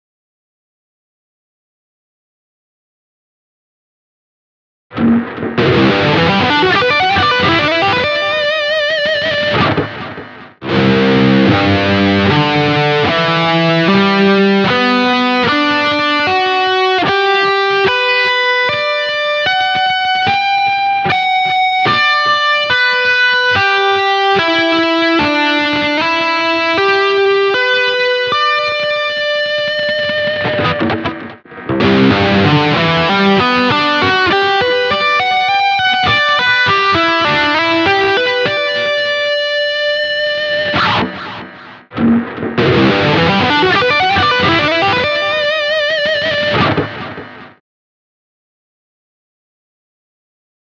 AZLicks #10 (8-string)
8-string tapping arpeggio lick on 16th notes on a Cmaj9#11 chord.